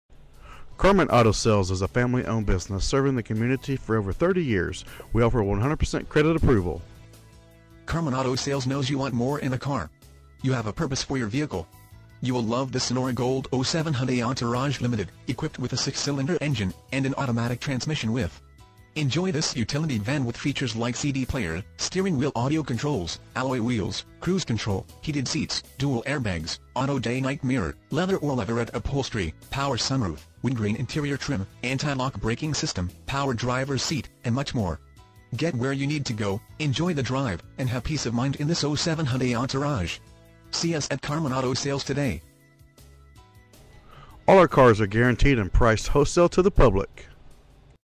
Hyundai Entourage, Karman Auto Sales- Lowell, MA 01851
Hyundai Entourage, Karman Auto Sales Karman Auto Sales knows you want more than just a car. you have a purpose for your vehicle. imagine driving this Sonora Gold 2007 Hyundai Entourage, equipped with a 6 Cyl. engine and an automatic transmission with 98,717 miles. enjoy this utility van with features like cd player, steering wheel audio controls, alloy wheels, cruise control, heated seats, dual air bags, auto Day Night mirror, leather or leatherette upholstery, power sunroof, and much more. Get where you need to go, enjoy the drive, and have peace of mind in this 2007 Hyundai Entourage. see us at Karman Auto Sales today!

miles- 98,717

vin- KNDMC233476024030

Karman Auto Sales

1418 Middlesex ST.

Lowell, Massachusetts 01851